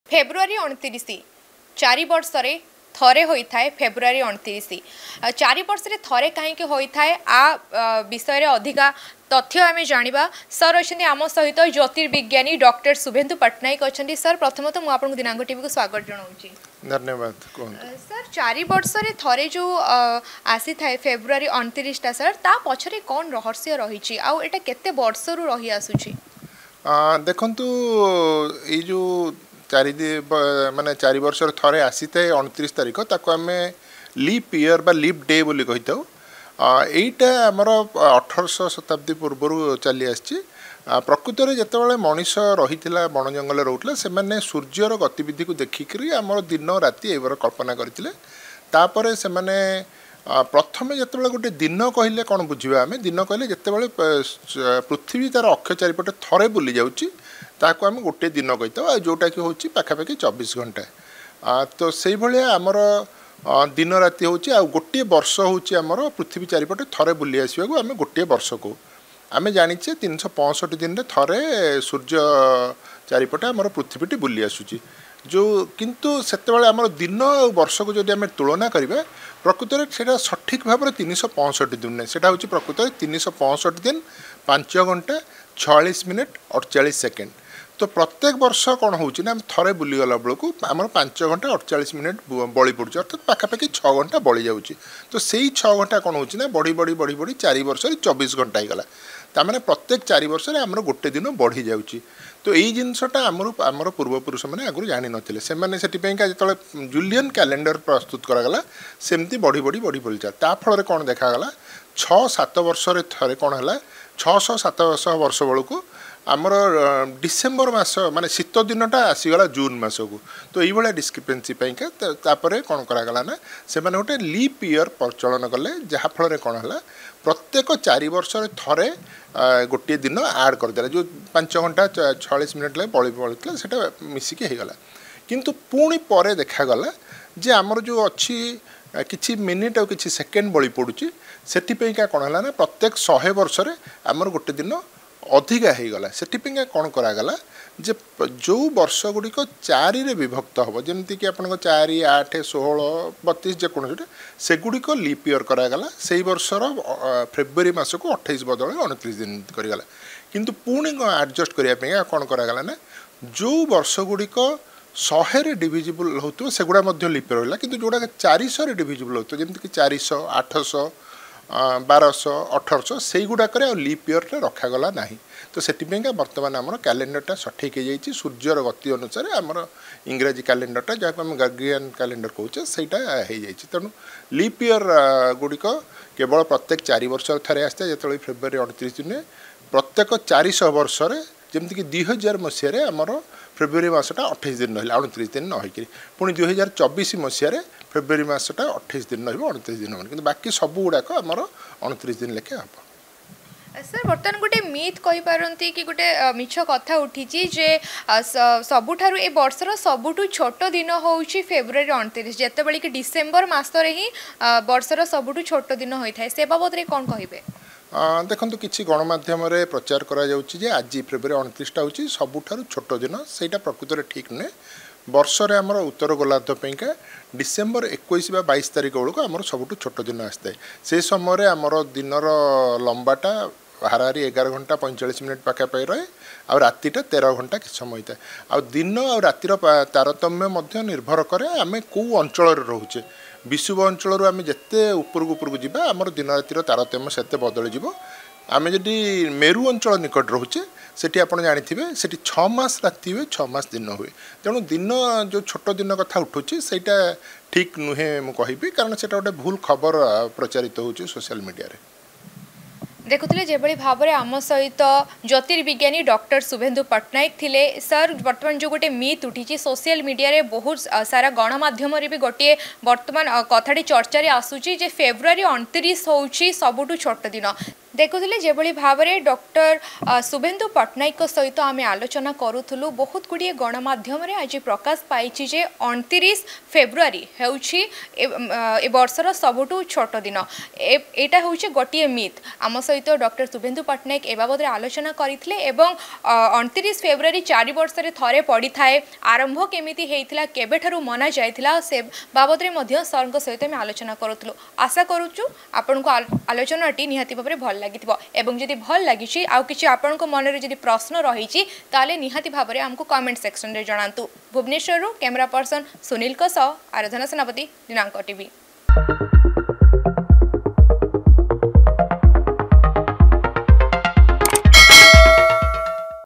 ଫେବୃଆରୀ 29 ର ରହସ୍ୟ | Dinanka Tv | February 29
ଫେବୃଆରୀ ୨୯ ଏହା ୪ ବର୍ଷରେ ଥରେ ପଡି ଥାଏ. କିନ୍ତୁ ଏହାର କାରଣ କଣ? କଣ ରହିଛି ଏହାର ରହସ୍ୟ? ଏହା ଜାଣିବା ପାଇଁ ଜ୍ୟୋତିବିଜ୍ଞାନୀ ଶୁଭେନ୍ଦୁ ପଟନାୟକଙ୍କ ସହ ଆଲୋଚନା